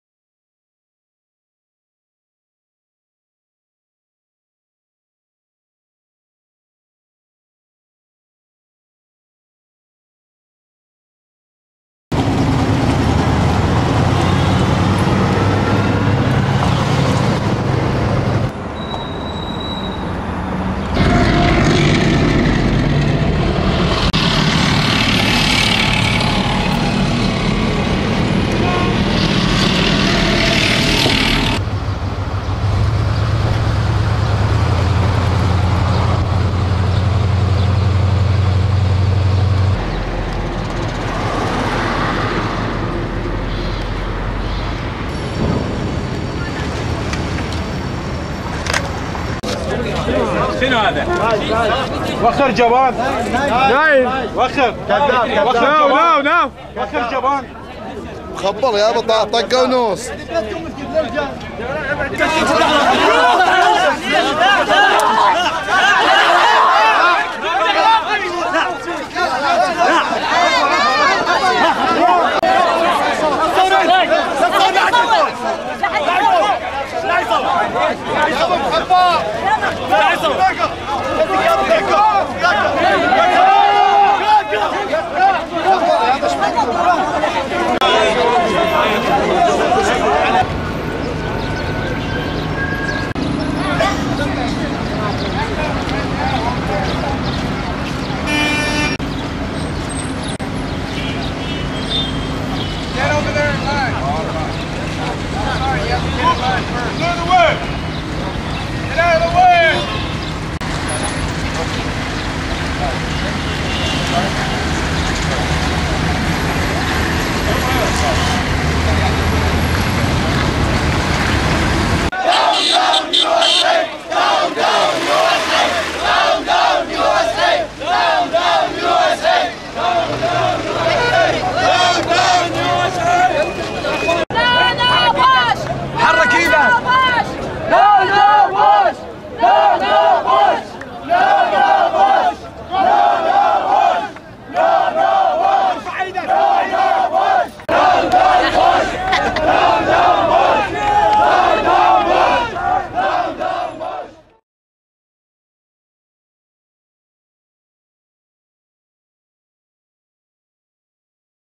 Arriving in Baghdad for the first time, April 2003.
My video of driving into Baghdad with my BBC colleagues for the first time after the 2003 Iraq war.
Febrile atmosphere, immediate public hostility to the Americans, US troops shouting at Iraqis in English, and a violent reaction to an Iraqi soldier trying to surrender.